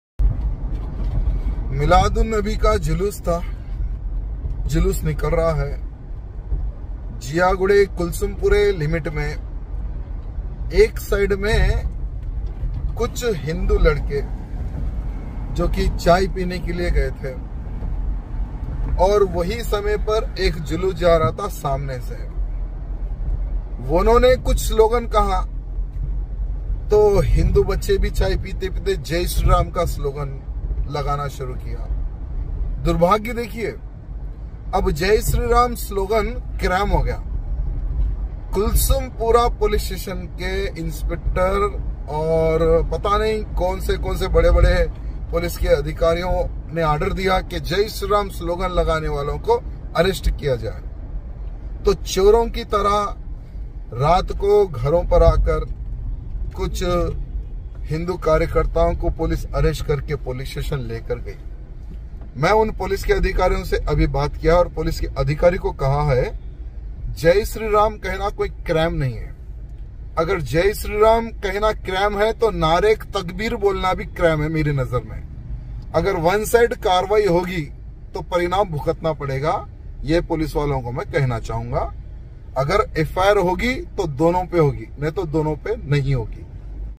Jiaguda Gopi Hotel ke paas Sharpasand anaasar ki jaanib se Milad Juloos mein Shareek hone wa
Jiaguda Gopi Hotel ke paas Sharpasand anaasar ki jaanib se Milad Juloos mein Shareek hone waale logon ke saamne naare lagaane ka waaqiya Jispe Goshamahal Kay Sadak Chap Mla Raja Singh Na Kaha Jai Sri Ram Bolne Jurm Hai Tow Narak Takbeer Bolna Bhi Jurm Hai